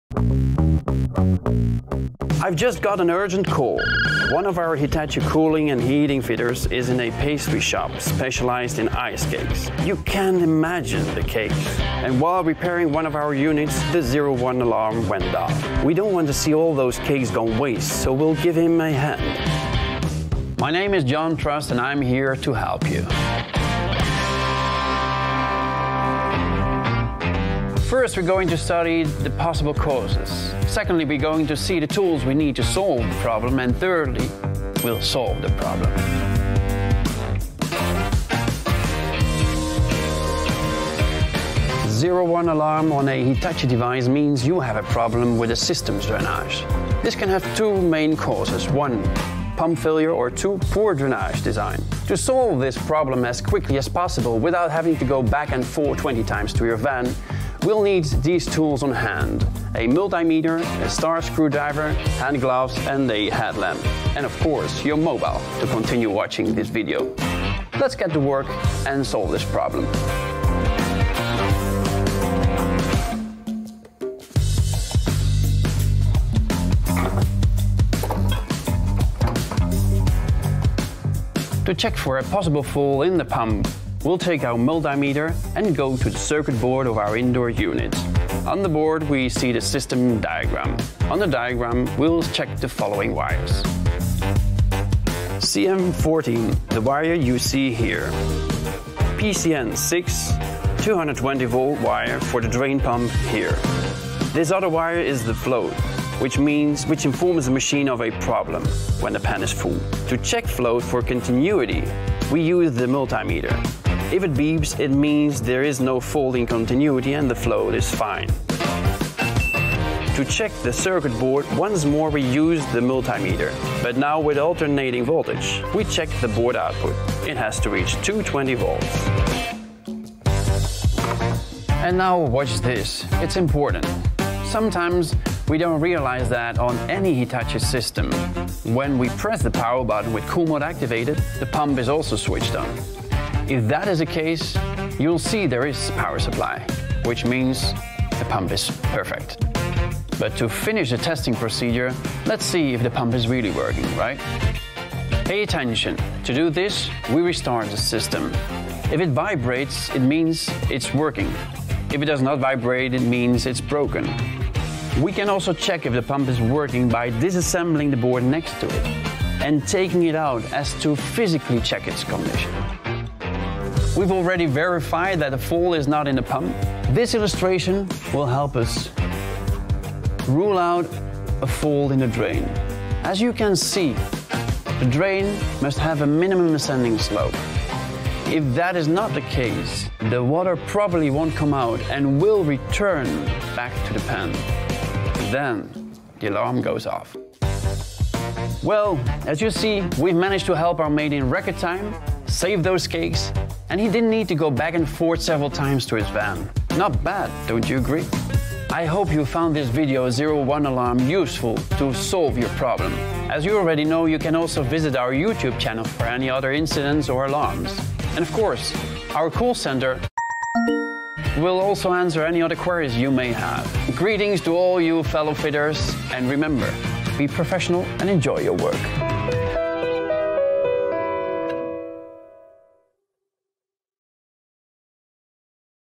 How to solve Hitachi air conditioner 01 Error Code on VRF Systems, Centrifugal VRF and Utopia
In this episode of The Solvers, learn how to fix Hitachi Commercial air conditioning Code 01.

So what does it mean?
An Alarm Code 01 on a Hitachi unit means there is a problem with the system's drainage. There can be two main causes for this issue:
1. Pump failure
2. Poor drainage design

In this video, we take you through the causes and solutions of an alarm code 01 for Centrifugal VRF, VRF and Utopia units.

Have The Solvers helped you fix and resolve the error?